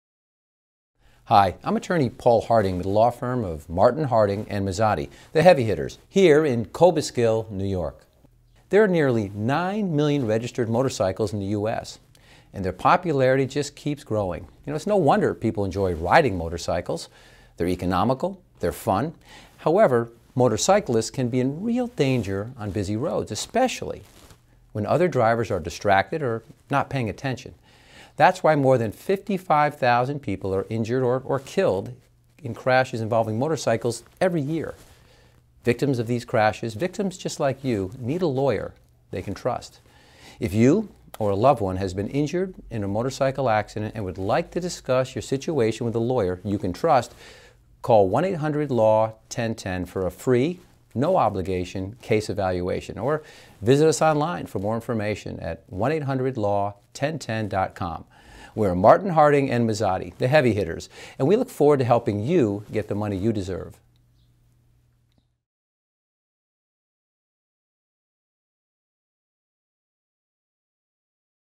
Cobleskill New York Motorcycle Attorney
Video Transcription:

I’m attorney Paul Harding with the law firm Martin, Harding and Mazzotti - the Heavy Hitters…
Here in Cobleskill, New York.
There are nearly 9 million registered motorcycles in the US. And their popularity just keeps growing.
It’s no wonder people enjoy riding motorcycles. They’re economical, and they’re fun!
However, motorcyclists can be in real danger on our busy roads, especially when other drivers are distracted or not paying attention.
That’s why more than 55,000 people are injured or killed in crashes involving motorcycles every year.
Victims of these crashes, victims just like you, need a lawyer they can trust.
If you or a loved one has been injured in a motorcycle accident and would like to discuss your situation with a lawyer you can trust…
Call 1800LAW1010 for a free, no obligation case evaluation…
We’re Martin, Harding and Mazzotti – the Heavy Hitters.
And we look forward to helping you get the money you deserve.